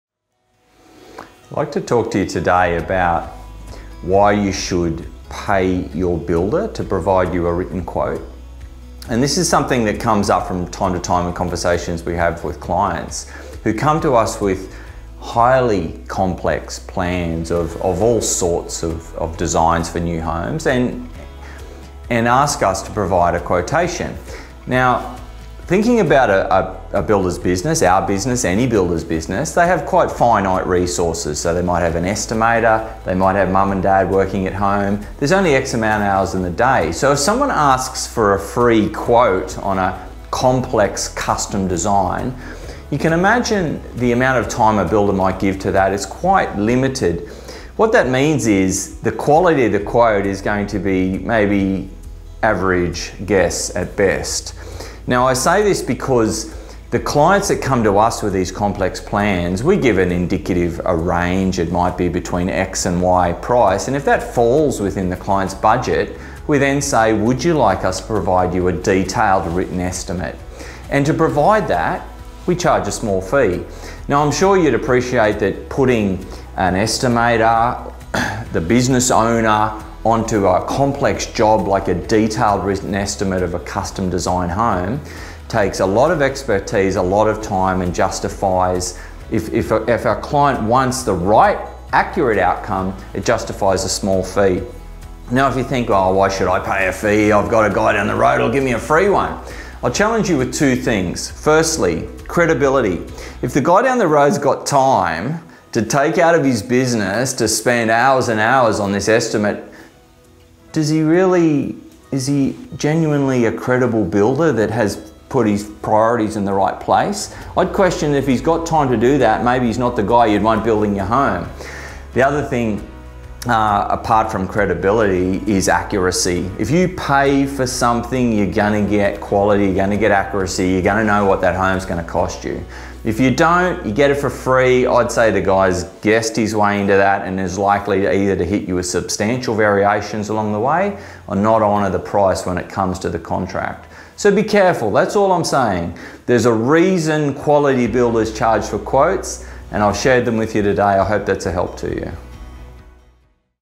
Should you pay your builder for a quote?
When embarking on a construction project, one of the crucial decisions you'll make is choosing the right builder. Among the factors to consider is whether they provide a comprehensive and accurate written quote. In this article, we'll delve into why paying for a written quote from your builder is essential. While some may be enticed by the offer of a free estimate, we'll discuss the limitations of such quotes and highlight the benefits of investing in a detailed, written estimate.